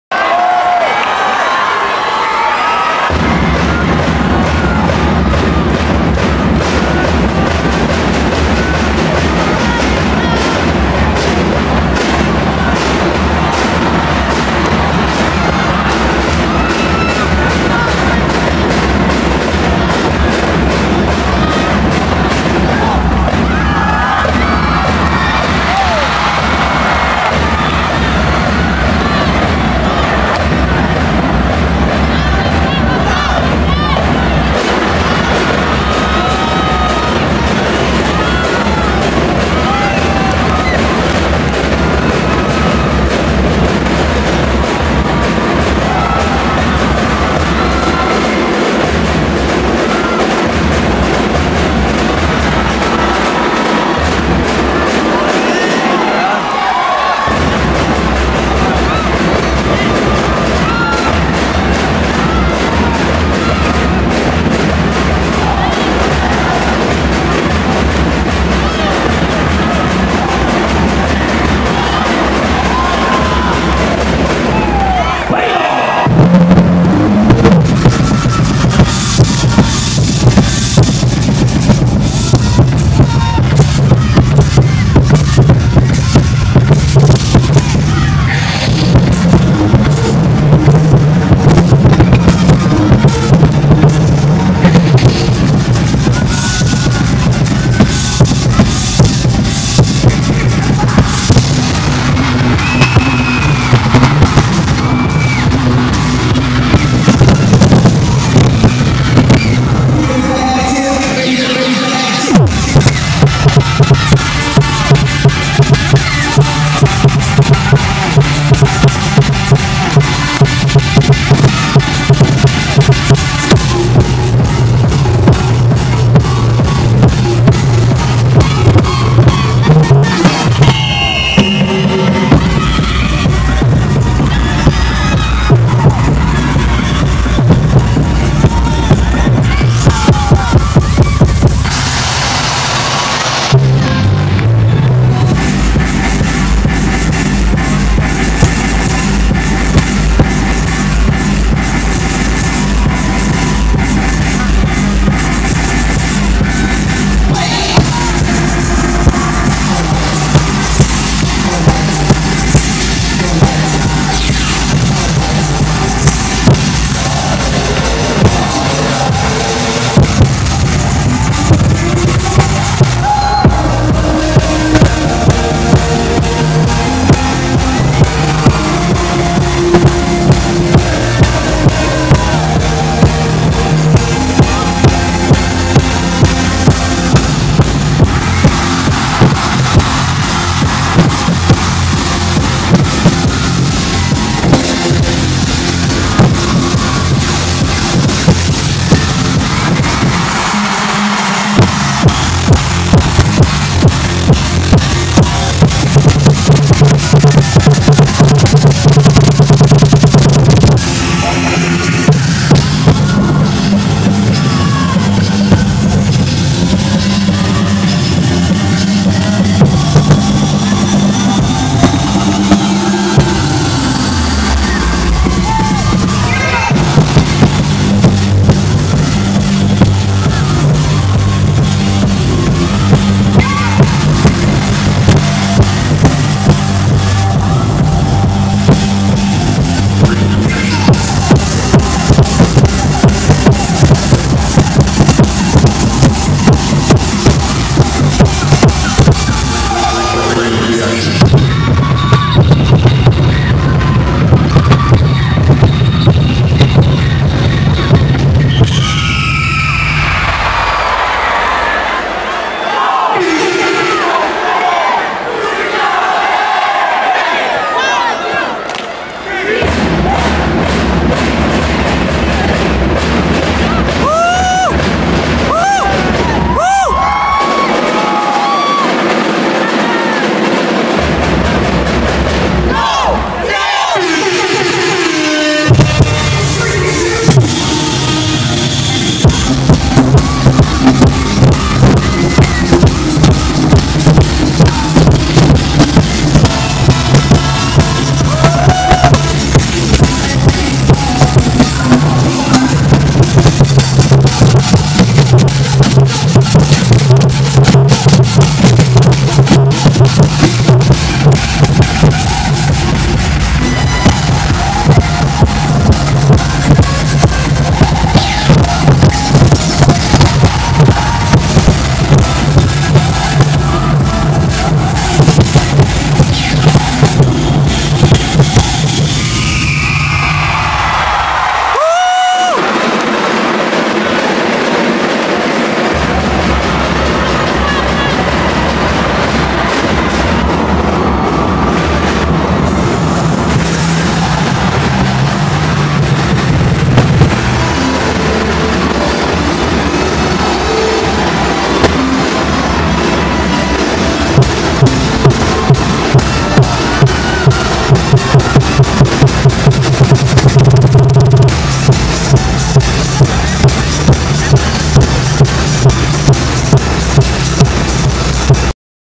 UAAP 76 Cheerdance - Ateneo Blue Babble Battalion
Ateneo Blue Babble Battalion performance at UAAP 76 Samsung Cheerdance Competition, September 15 at MOA Arena